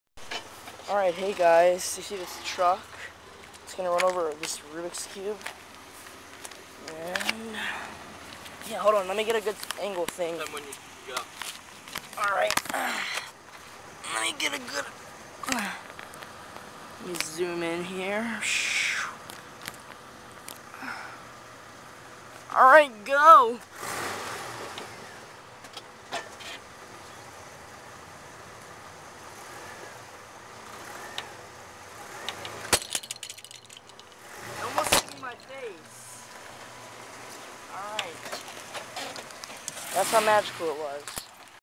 4x4 Truck vs Magic Cube!!!
yess i went to the limit killing the worlds shitties rubiks cube out there